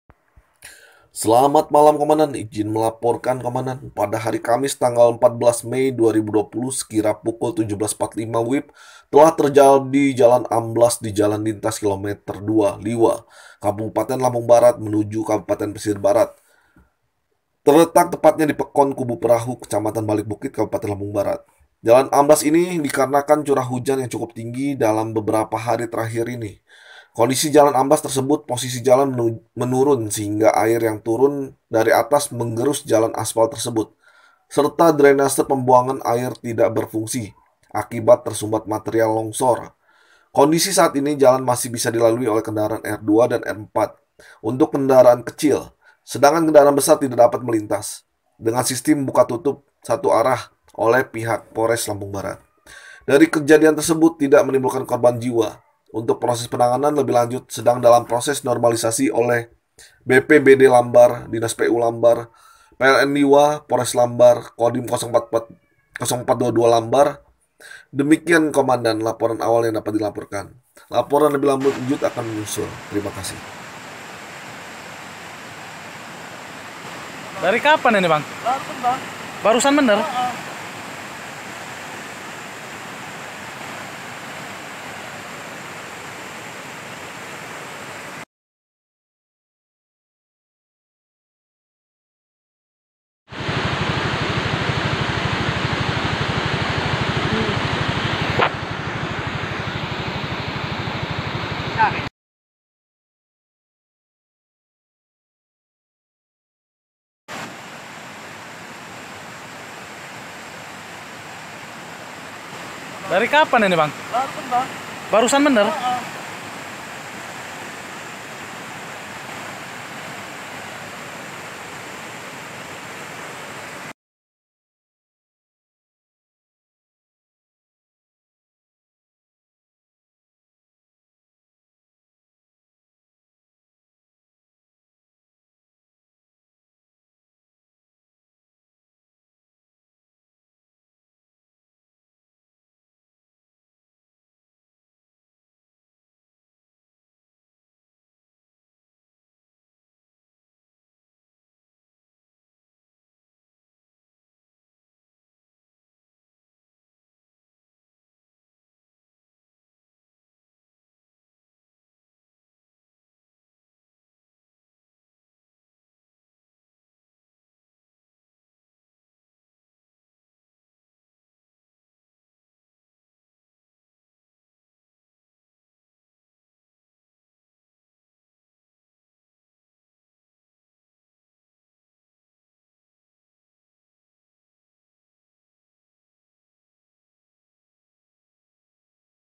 tanah Longsor Di jalan Km 32 Liwa Lampung Barat
Kejadian tanah longsor, di Jalan  Di Km 32 Liwa Lampung Barat